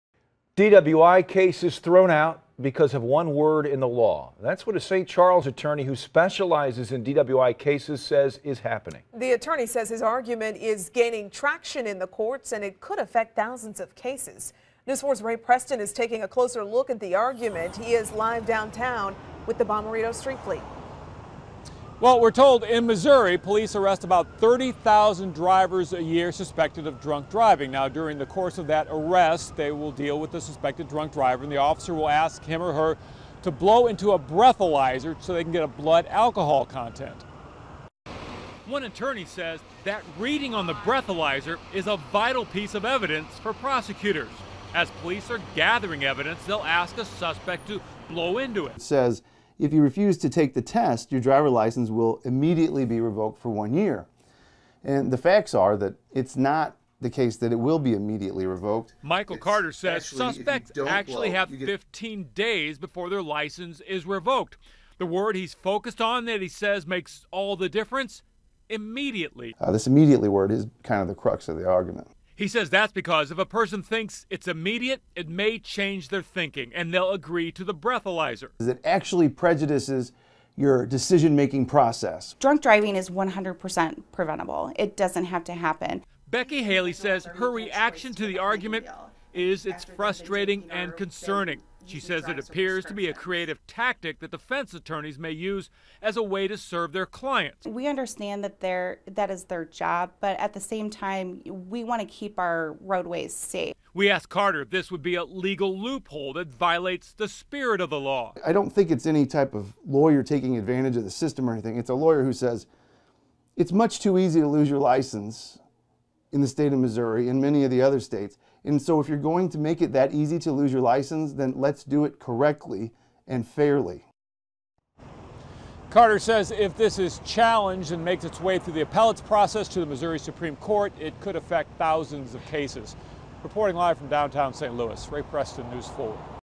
DidYouBlow.com DWI Attorney Mike Carter: One word in law could cause DWI cases to be tossed MISSOURI
DWI defense attorney Michael Carter (founder of DidYouBlow.com) says a Breathalyzer test is one valuable piece of evidence in a DWI case. Carter said a law dealing with DWIs in Missouri says if a suspected drunk driver refuses to take a Breathalyzer test, he or she will 'immediately' lose their license for a year.
However, Carter tells News 4 suspects have 15 days before their license is revoked, adding what the word 'immediately' means is the center of  debate. Carter says if a suspected drunk driver were to lose their license right away, it would change their thinking on the Breathalyzer test.